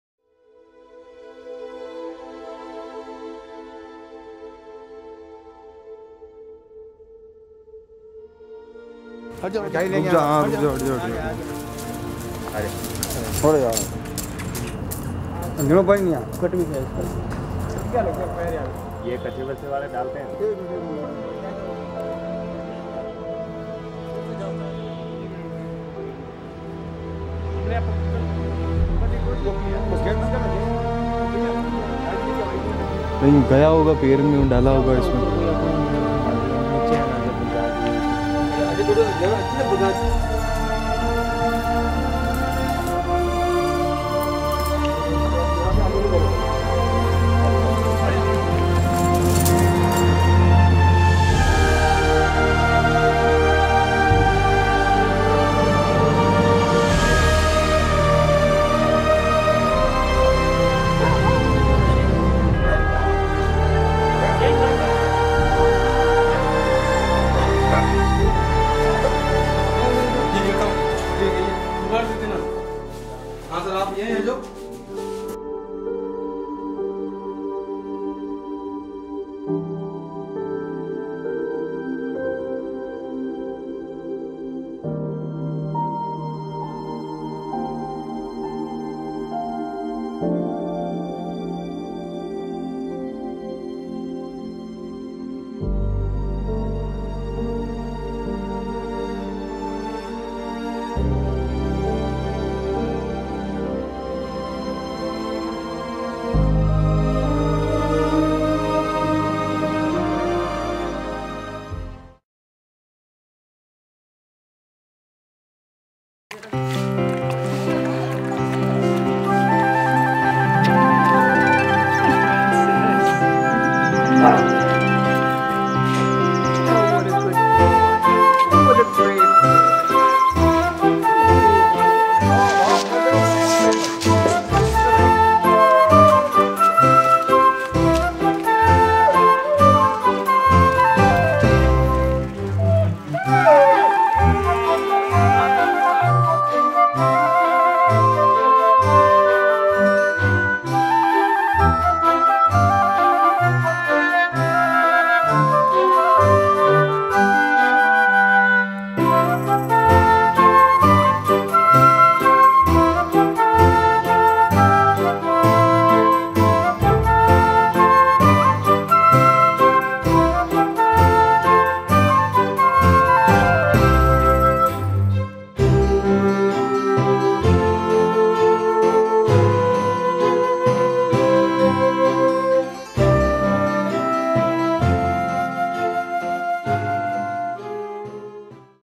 Street dog's foot caught in deadly trap. Watch his rescue with a tissue!
Animal Aid's helpline received a call reporting a dog whose paw was caught in an animal trap used to kill rabbits and other small animals. We had one chance to catch him to save his life.